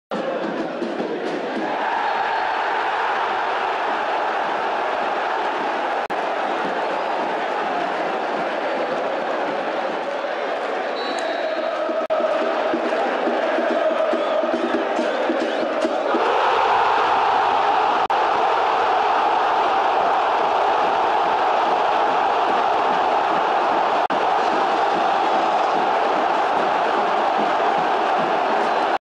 Locura en la tribuna en gol de Alario (sonido ambiente) *Chequeen a Driussi
En una calurosa tarde (5/Nov/2016) en el estadio de Huracán, en donde River hizo de local ante Estudiantes, el Millonario empató 1  a 1 con el Pincha